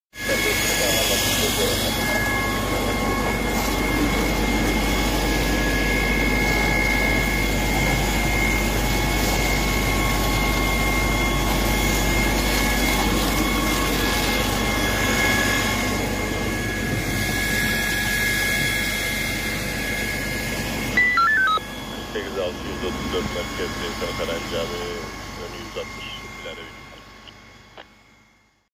Kahramanmaraş’ta otomobil seyir halinde alev aldı
Kahramanmaraş’ta seyir halinde iken alev alan otomobil hurdaya döndü.